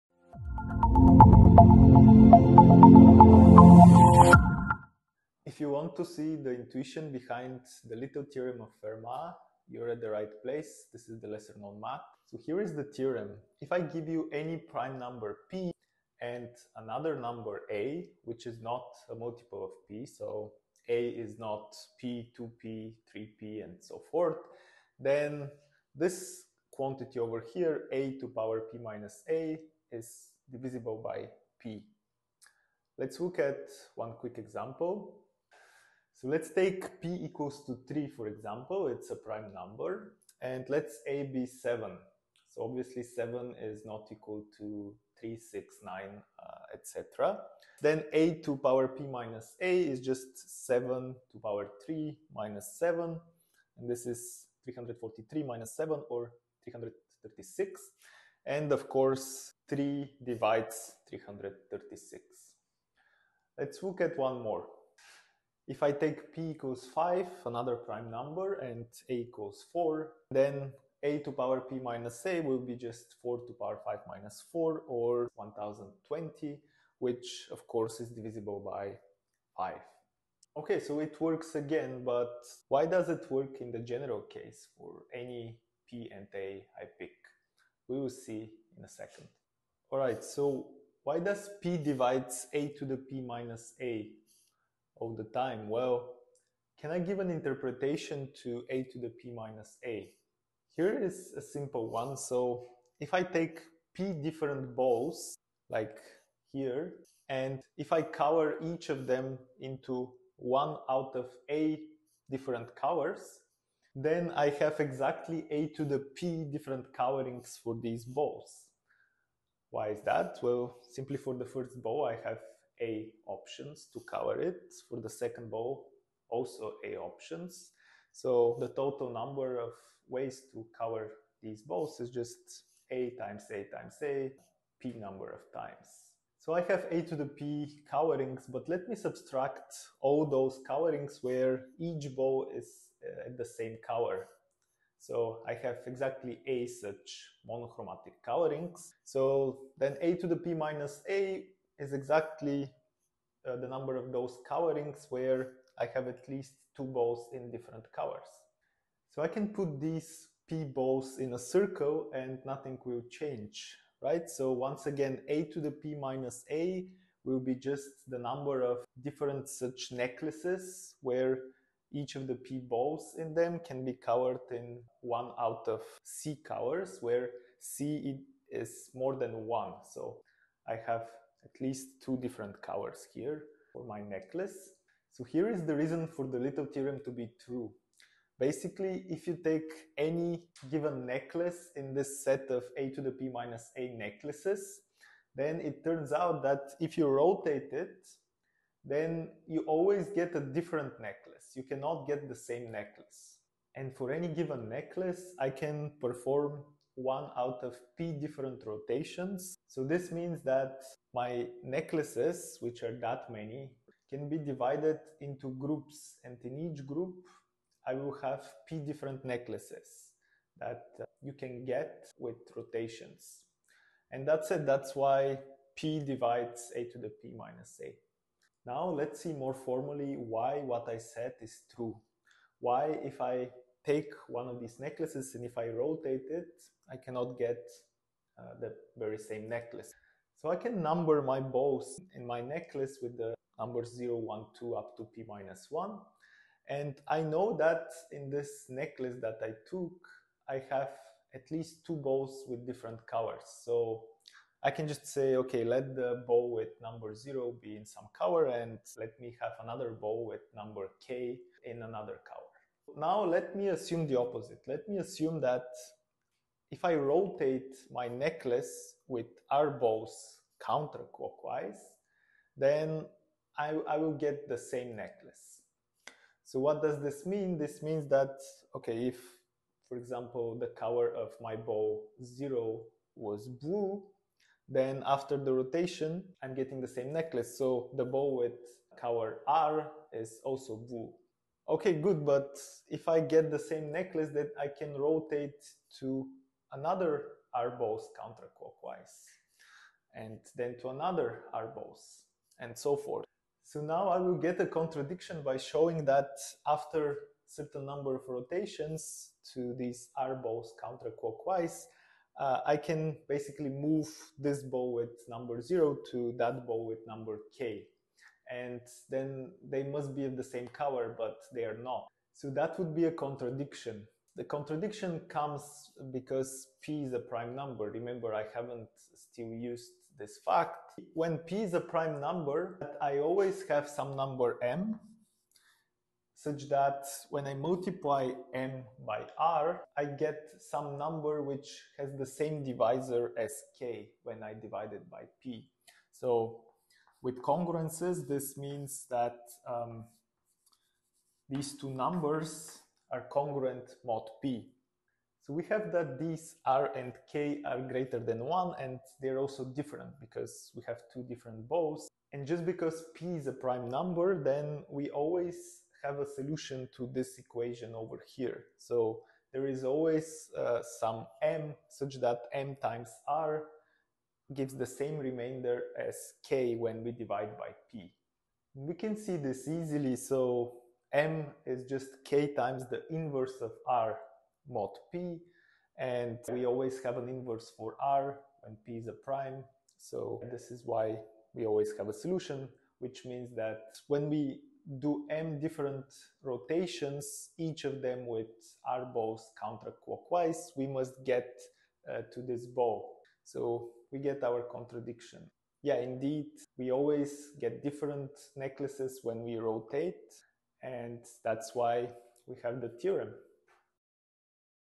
Fermat's Little Theorem - Simple Intuitive Proof
A proof that helps us to understand Fermat’s Little Theorem!

Fermat’s Little Theorem (to not be confused with Fermat's Last Theorem)

TIMESTAMPS

0:11 The Theorem + examples
1:29 Cool interpretation of a^p - a
3:04 The reason for the Little Theorem to be true
3:48 Why rotating always gives a different necklace?